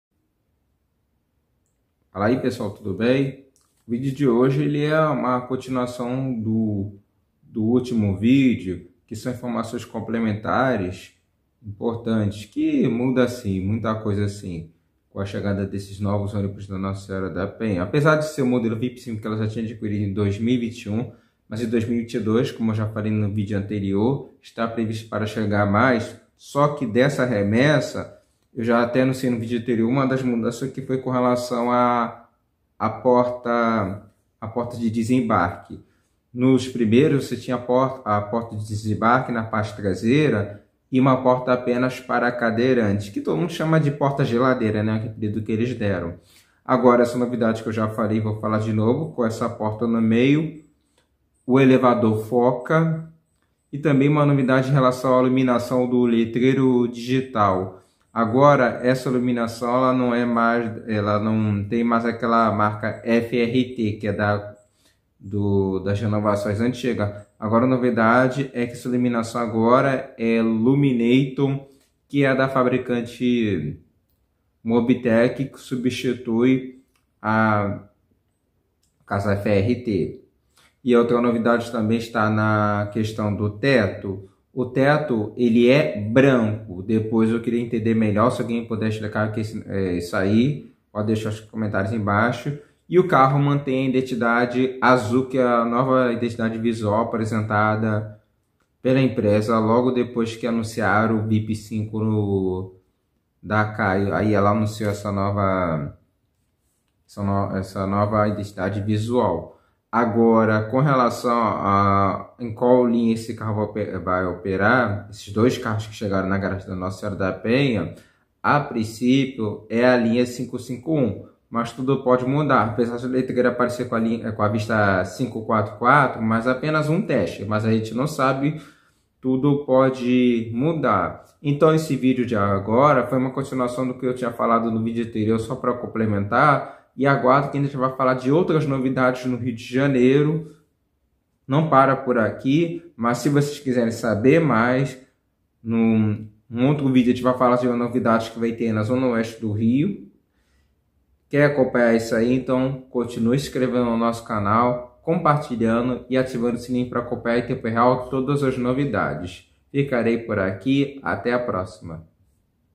Outras mudanças na configuração dos novos Vip V da Nossa Senhora da Penha
Essas mudanças envolvendo iluminação, acabamentos e o elevador.